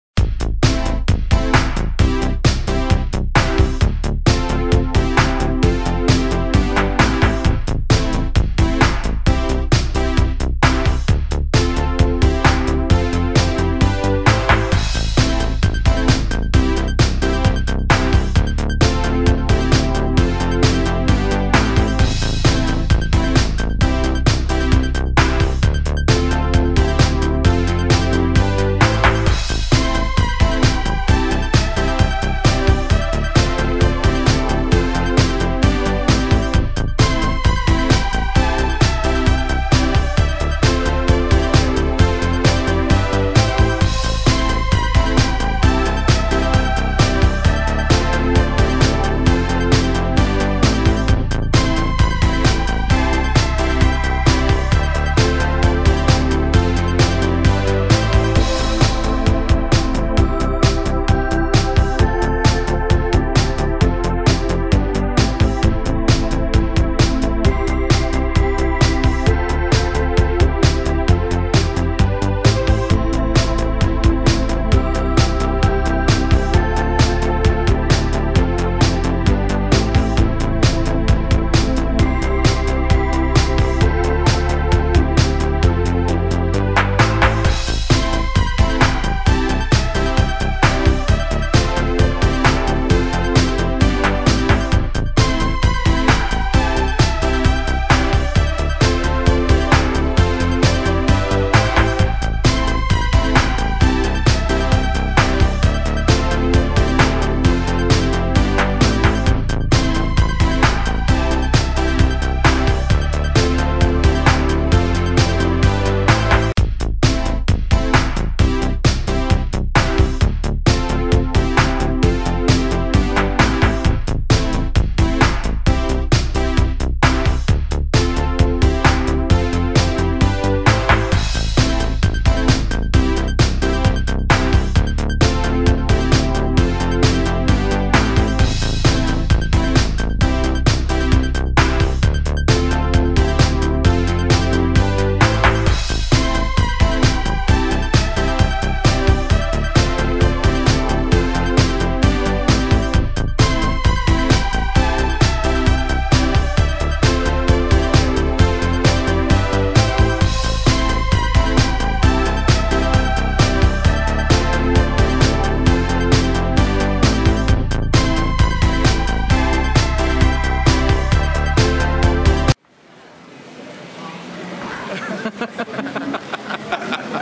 4. Retrobörse Saar | FourYou unterwegs mit dem Lautschmer #1
Am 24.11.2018 waren der Paul und Flo zusammen mit dem Markus aka Lautschmer auf der Retrobörse Saar. Was sie da gesehen haben und was es da alles an Retrozeug gab seht ihr hier. Viel Spaß :)